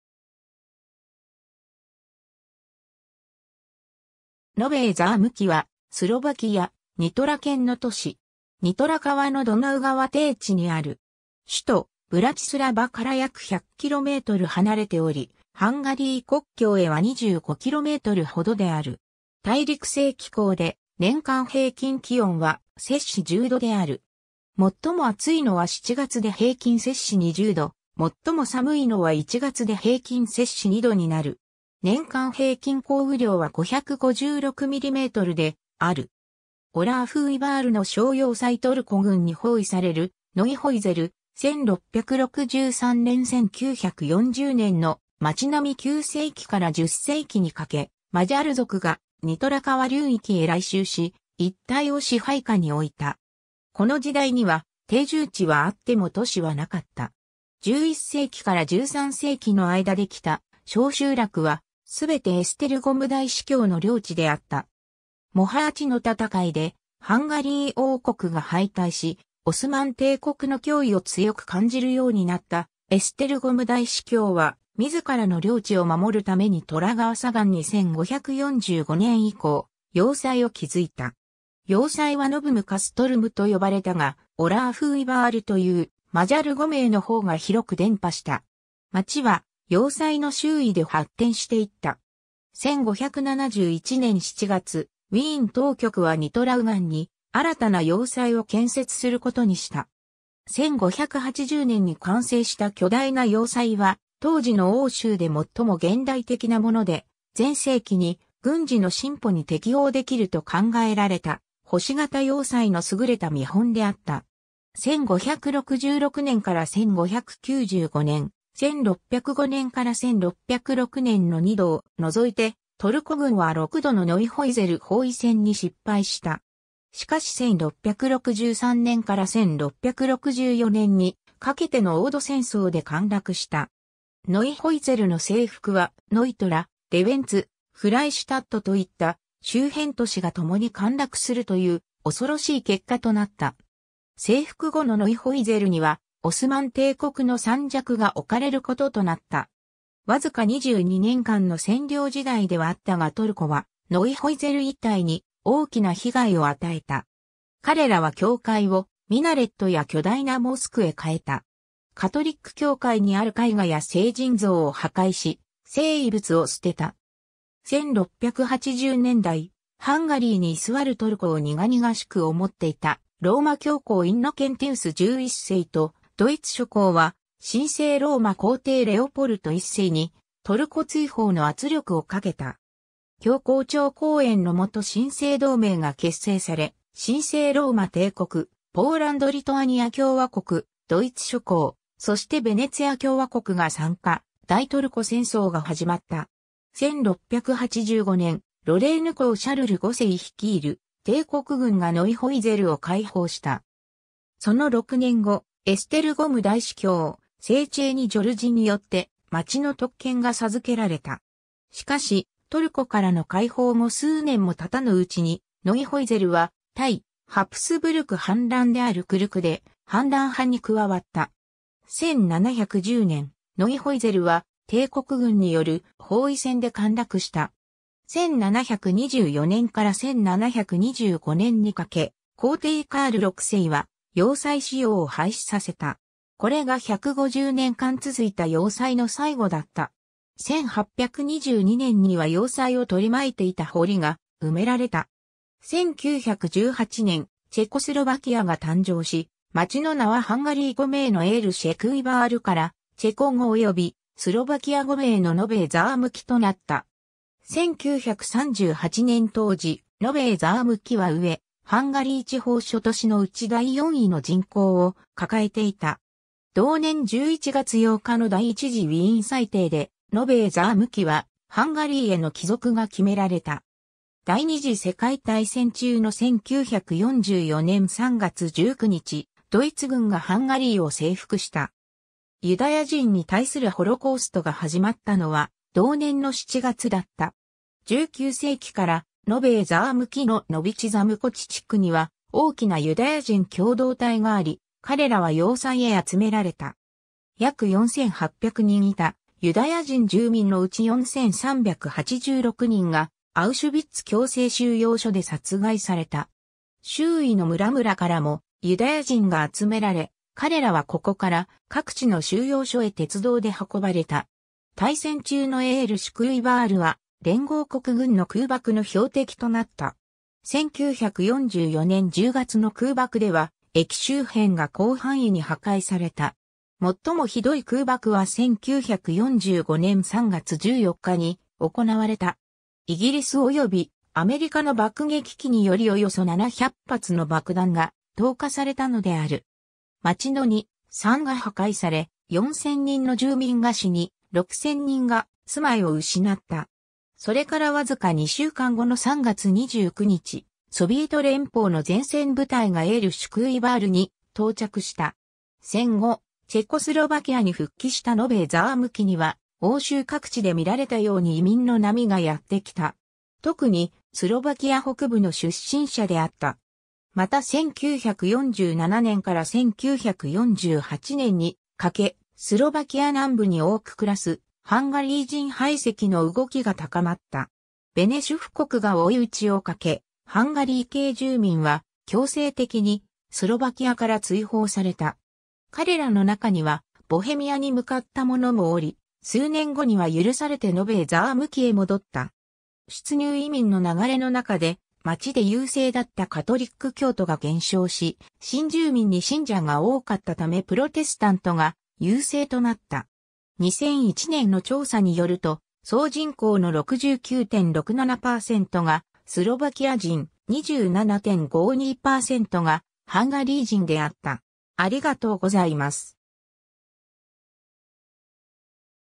ノヴェー・ザームキ
ノヴェー・ザームキ （スロバキア語:Nové Zámky、マジャル語:Érsekújvárエールシェクーイヴァール、ドイツ語:Neuhäus[e]lノイホイゼル、トルコ語:Uyvar、ラテン語:Novum Castrum）は、スロバキア、ニトラ県の都市。
 ニトラ川のドナウ川低地にある。
首都ブラチスラヴァから約100km離れており、ハンガリー国境へは25kmほどである。
 大陸性気候で、年間平均気温は10℃である。
最も暑いのは7月で平均20℃、最も寒いのは1月で平均－2℃になる。
年間平均降雨量は556mmである。
 オラーフーイヴァールの小要塞 トルコ軍に包囲されるノイホイゼル、1663年 1940年の町並み 9世紀から10世紀にかけ、マジャル族がニトラ川流域へ来襲し、一帯を支配下においた。
この時代には定住地はあっても都市はなかった。
11世紀から13世紀の間できた小集落は、すべてエステルゴム大司教の領地であった。
 モハーチの戦いでハンガリー王国が敗退し、オスマン帝国の脅威を強く感じるようになったエステルゴム大司教は、自らの領地を守るためニトラ川左岸に1545年以降要塞を築いた。
要塞はノヴム・カストルムと呼ばれたが、オラーフーイヴァール（Oláhújvár）というマジャル語名の方が広く伝播した。
町は要塞の周囲で発展していった。
1571年7月、ウィーン当局はニトラ右岸に新たな要塞を建設することにした。
1580年に完成した巨大な要塞は当時の欧州で最も現代的なもので、前世紀に軍事の進歩に適応できると考えられた星型要塞の優れた見本であった。
 1566年から1595年、1605年から1606年の2度を除いて、トルコ軍は6度のノイホイゼル包囲戦に失敗した。
しかし1663年から1664年にかけての墺土戦争（en）で陥落した。
ノイホイゼルの征服は、ノイトラ（ニトラ）、レヴェンツ（レヴィツェ）、フライシュタット（フロホヴェツ）といった周辺都市がともに陥落するという恐ろしい結果となった。
征服後のノイホイゼルにはオスマン帝国のサンジャク（県）が置かれることとなった。
わずか22年間の占領時代ではあったがトルコはノイホイゼル一帯に大きな被害を与えた。
彼らは教会を、ミナレットや巨大なモスクへ変えた。
カトリック教会にある絵画や聖人像を破壊し、聖遺物を捨てた。
 1680年代、ハンガリーに居座るトルコを苦々しく思っていたローマ教皇インノケンティウス11世とドイツ諸侯は、神聖ローマ皇帝レオポルト1世にトルコ追放の圧力をかけた。
教皇庁後援のもと神聖同盟が結成され、神聖ローマ帝国、ポーランド・リトアニア共和国、ドイツ諸侯、そしてヴェネツィア共和国が参加、大トルコ戦争が始まった。
1685年、ロレーヌ公シャルル5世率いる帝国軍がノイホイゼルを解放した。
その6年後、エステルゴム大司教セーチェーニ・ジョルジによって町の特権が授けられた。
 しかしトルコからの解放後数年もたたぬうちに、ノイホイゼルは対ハプスブルク反乱であるクルク（en）で反乱派に加わった。
1710年、ノイホイゼルは帝国軍による包囲戦で陥落した。
1724年から1725年にかけ、皇帝カール6世は要塞使用を廃止させた。
これが150年間続いた要塞の最後だった。
1822年には要塞を取り巻いていた堀が埋められた。
 1918年、チェコスロバキアが誕生し、町の名はハンガリー語名のエールシェクーイヴァールからチェコ語およびスロバキア語名のノヴェー・ザームキとなった。
 1938年当時、ノヴェー・ザームキは上ハンガリー地方（hu）諸都市のうち第4位の人口を抱えていた。
同年11月8日の第一次ウィーン裁定で、ノヴェー・ザームキはハンガリーへの帰属が決められた。
第二次世界大戦中の1944年3月19日、ドイツ軍がハンガリーを征服した。
ユダヤ人に対するホロコーストが始まったのは同年の7月だった。
19世紀からノヴェー・ザームキのノヴィチ・ザムコチ地区（Nových Zámkoch）には大きなユダヤ人共同体があり、彼らは要塞へ集められた。
約4800人いたユダヤ人住民のうち4386人がアウシュヴィッツ強制収容所で殺害された。
周囲の村々からもユダヤ人が集められ、彼らはここから各地の収容所へ鉄道で運ばれた。
 大戦中のエールシュクーイヴァールは、連合国軍の空爆の標的となった。
1944年10月の空爆では駅周辺が広範囲に破壊された。
最もひどい空爆は1945年3月14日に行われた。
イギリス及...